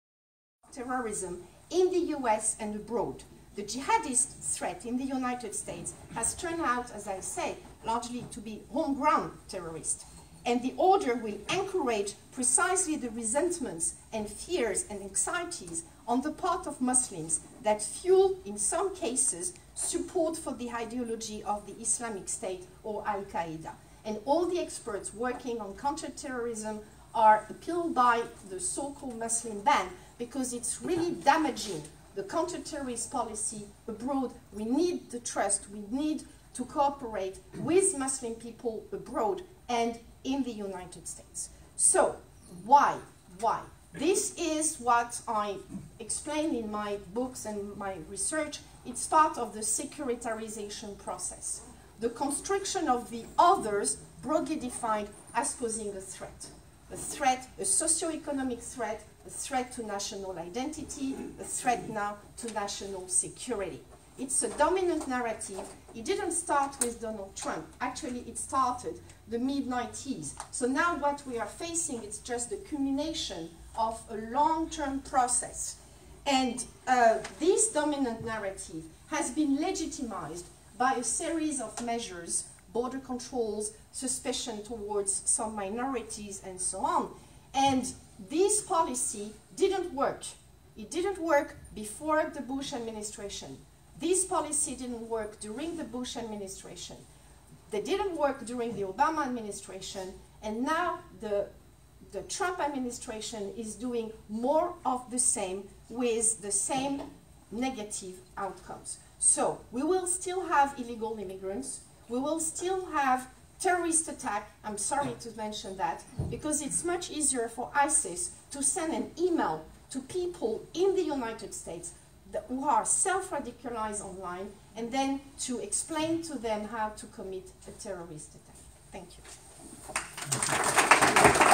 Implications of the Trump Presidency - Dr. Ariane Chebel d'Appollonia Remarks
SPAA Professor Dr. Ariane Chebel d'Appollonia says that homegrown terrorists pose more of a threat than individuals who would be prevented from entering the United States under the travel ban.